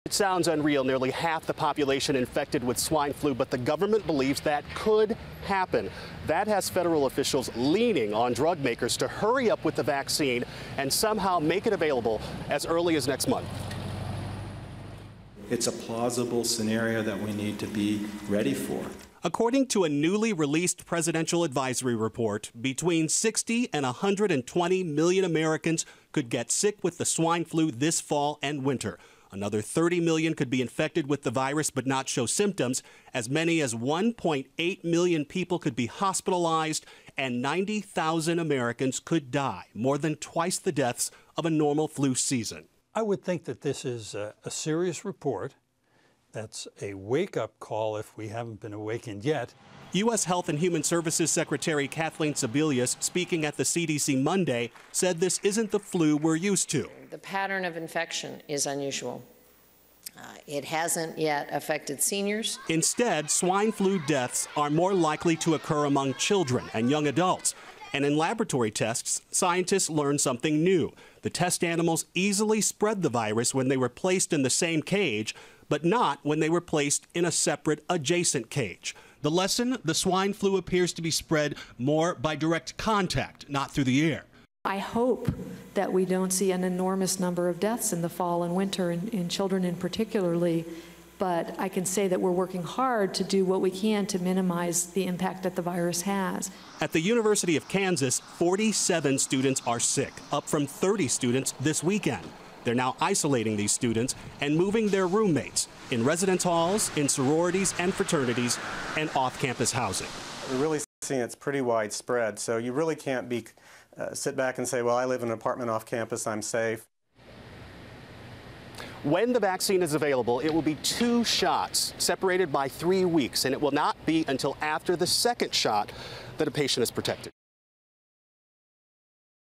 Swine Flu: Worst-Case Scenario
C.D.C reports half the country could become infected with swine flu.